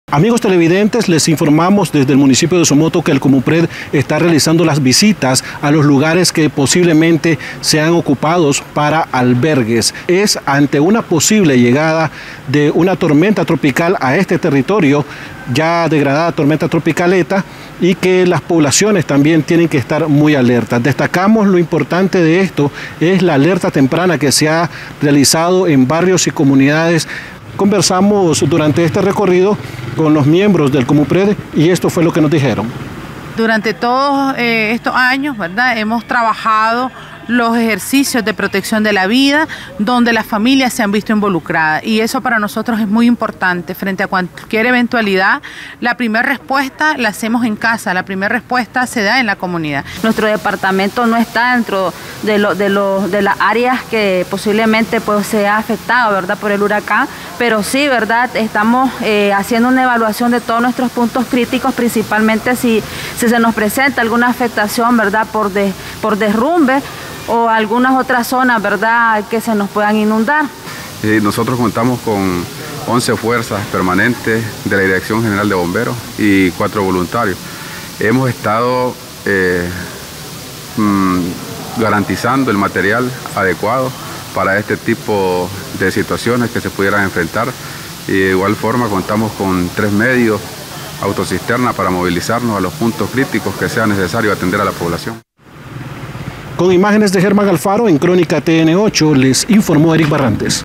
COMUPRED supervisa posibles albergues ante la amenaza de Eta en Somoto – Nicaragua
El comité municipal de prevención de desastres en el municipio de Somoto se mantiene activo ante la alerta verde decretada en el país por la llegada del huracán ETA.

Se han dispuesto a realizar visitas a los lugares donde se prevé crear sitios para albergar a personas que lo necesiten en caso de una posible tormenta tropical que deje varias horas de lluvias.

Cumpliendo con todas las medidas establecidas ante la amenaza del huracán por el sistema nacional de prevención de desastres (SINAPRED) han trabajado coordinadamente en un sistema integrado por Instituciones estatales.

Desde hace unos años se ha trabajado activamente en los ejercicios de protección a la vida donde los protagonistas son las familias de zonas con alto riesgo de deslaves a causa de constantes lluvias.

Por lo tanto es de conocimiento de las familias que en primera instancia deben refugiarse en casas solidarias, de ser necesario ocupar zonas de albergues establecidas por las autoridades y en tercer opción ocupar los centros escolares por disposición de las autoridades dependiendo de la magnitud de los desastres.

Se destaca la inclusión de organismos no gubernamentales dispuestos a formar parte del COMUPRED junto a las Instituciones de Gobierno para brindar ayuda a las familias que lo necesiten.

Prevención de desastres
“Estamos tomando las medidas, confiando en Dios, estamos transmitiendo a la población la calma, pero sobre todo estamos actuando de manera responsable y oportuna verificando nuestros albergues, la disponibilidad de recursos, de suministros, a la hora de una situación”, destacó Dania Martínez alcaldesa de Somoto.

También dijo que han visitado las gasolineras para conocer la disponibilidad del combustible para la movilidad de la población.

También han identificado zonas que podrían quedar aisladas por las crecidas de los ríos, cauces y posibles derrumbes como ha sucedido en las zonas rurales y barrios vulnerables.

Rosa Elena Alvarado, coordinadora del COMUPRED de Somoto indicó "a pesar de que Madriz no se encuentra en las áreas de mayor afectación, pero continuamos realizando la evaluación de todos los puntos críticos ante la amenaza de la que sería tormenta tropical al cruzar por el territorio de Madriz en las próximas horas".